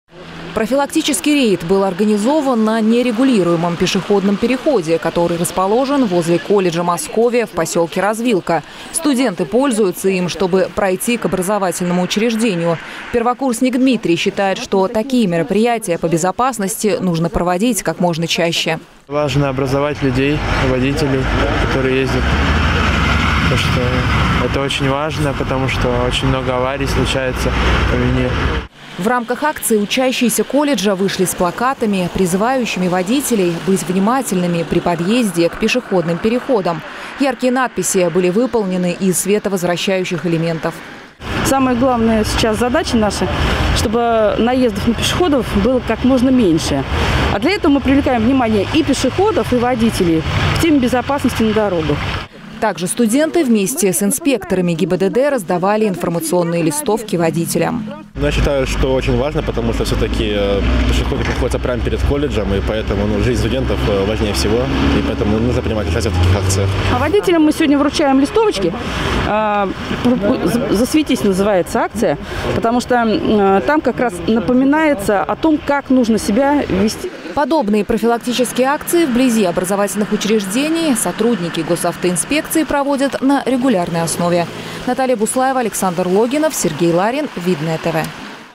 ОСТРОЖНО: ПЕШЕХОД!
Сотрудники Госавтоинспекции провели совместный рейд со студентами колледжа «Московия». В рамках профилактического мероприятия водителям и пешеходом напомнили о необходимости быть внимательными на проезжей части.